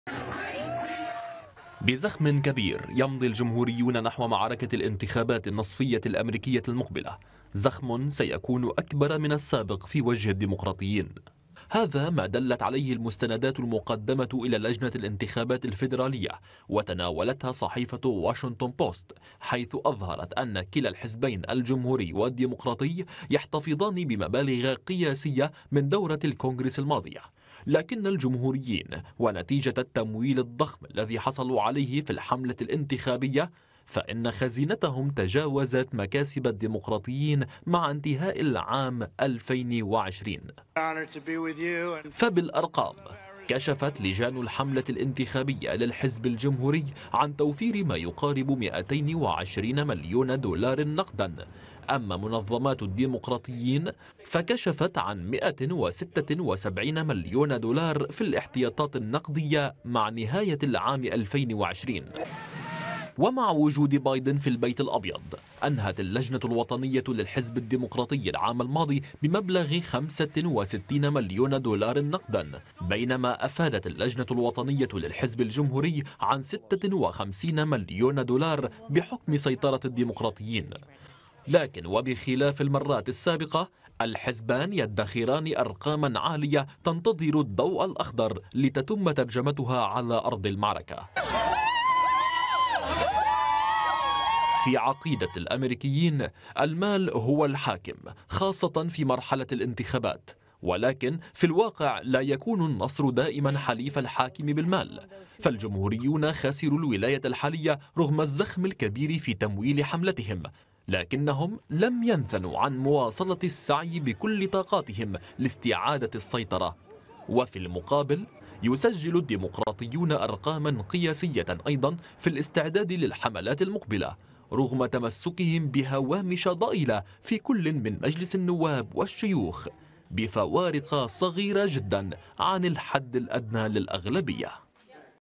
أرقام قياسية للتبرعات للانتخابات النصفية الأميركية مع تصدر الجمهوريين
إعداد: حبيب عزالدين